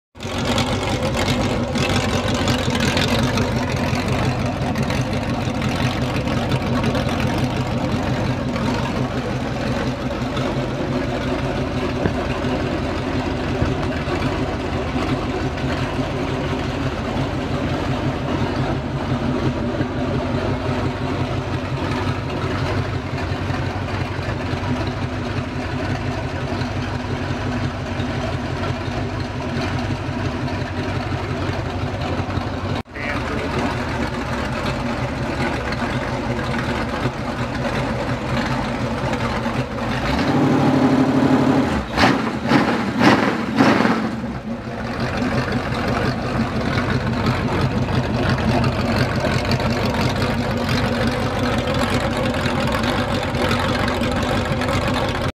421 motor
The 421 lives again. Fresh out of the shop. WH , Date code K135 (early 65), 4 speed, tri power, 4 bolt main. Missing the tripower, for now.